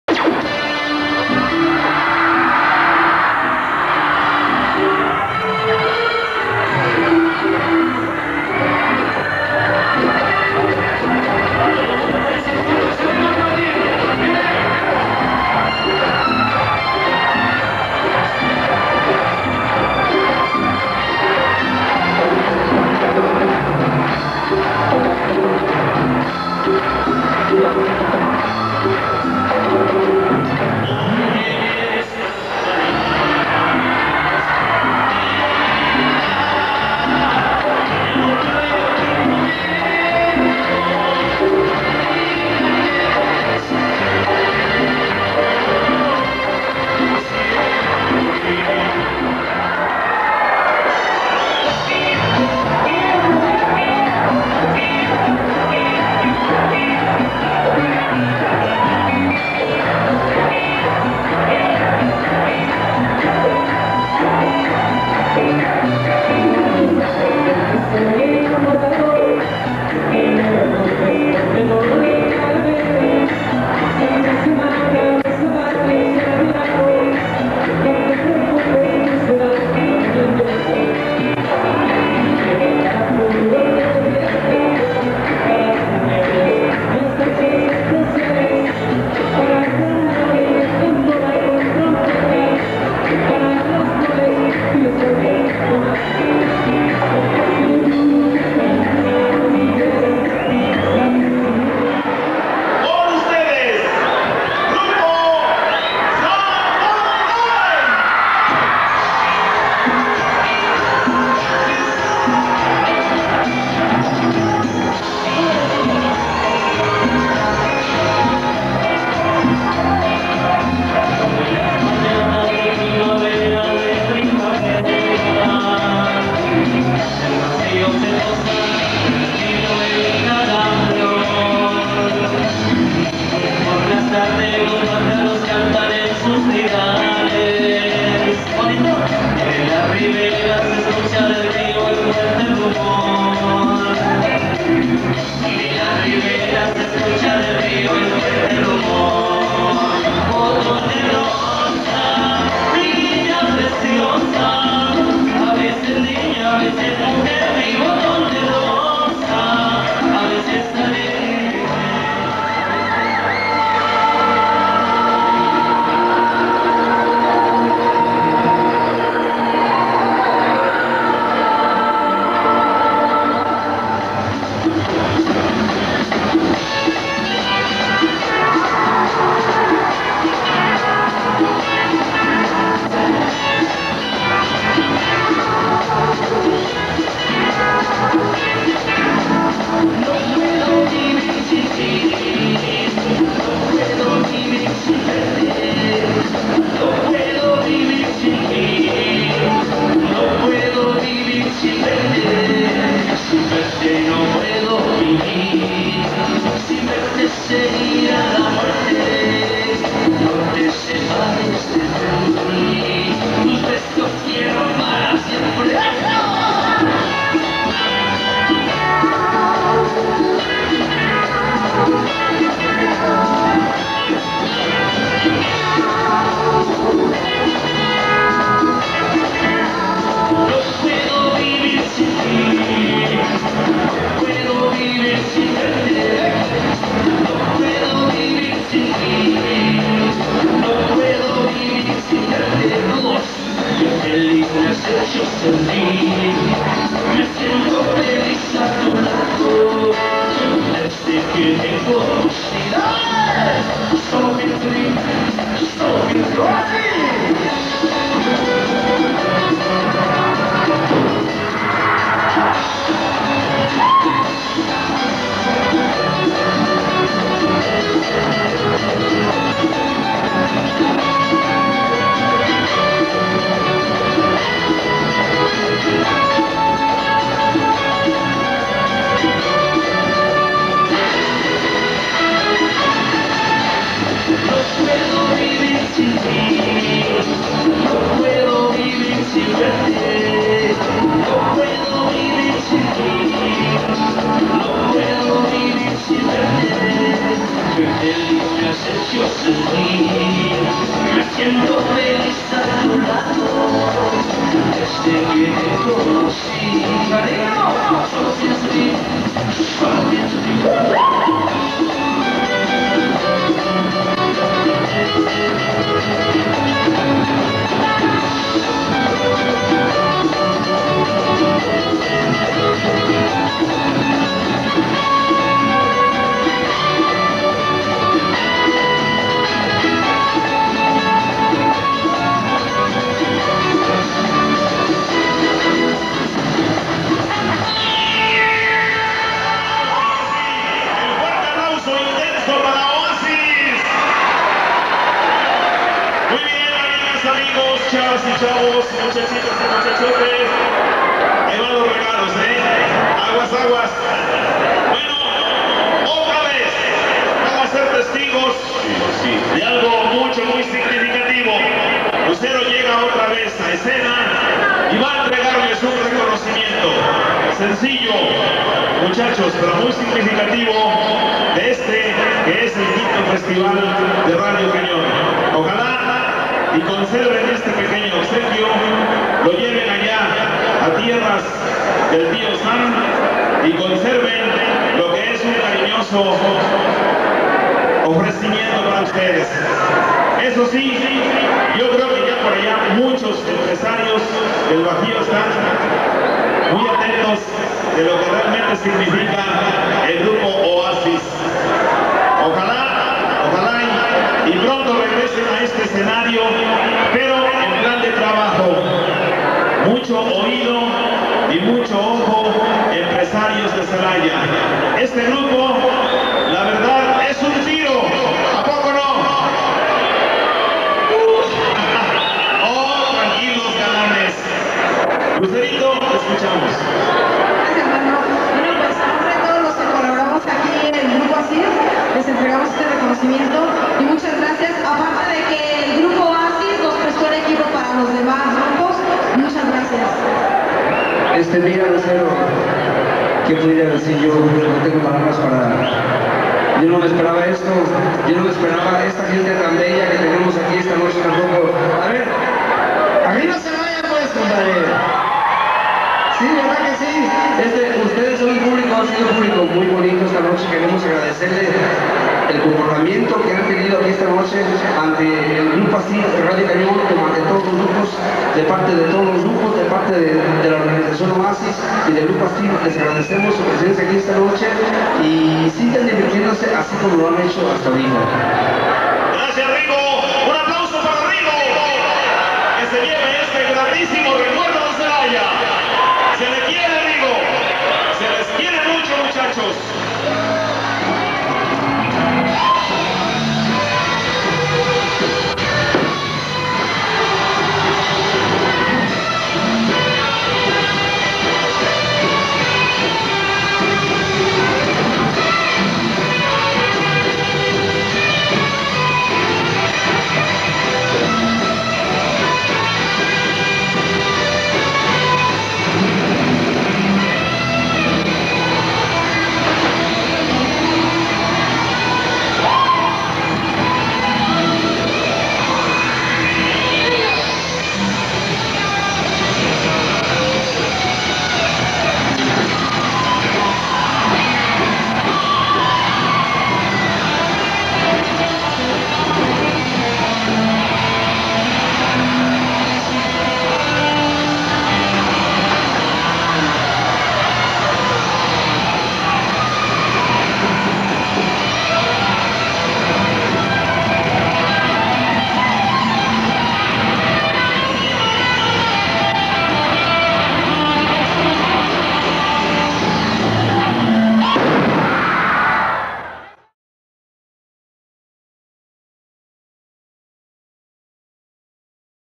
Oasis - Concert in Celaya Guanajuato Mex
Oasis in concert with "Los Acosta", "Los Rodarte", and "Samuray" in the Salon de Fiestas Rodeo, Celaya, Guanajuato - October 18,1990.